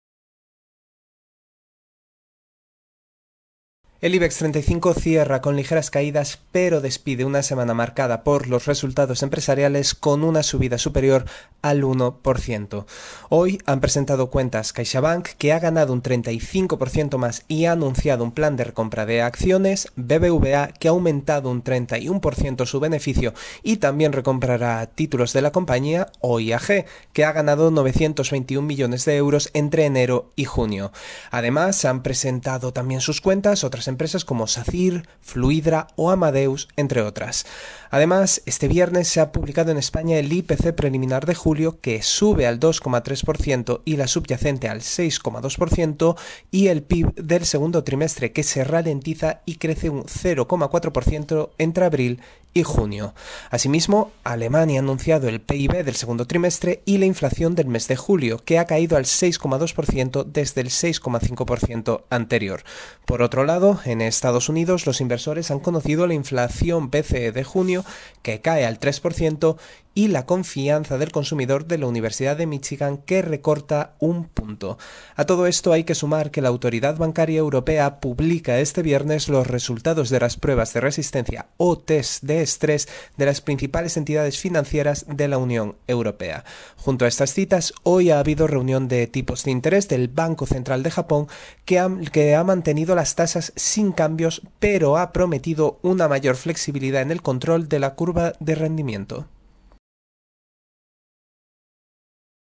El Ibex gana un 1,2% en una semana marcada por los resultados y otea los 10.000 puntos
Ibex 35: El Ibex gana un 1,2% en una semana marcada por los resultados y otea los 10.000 puntos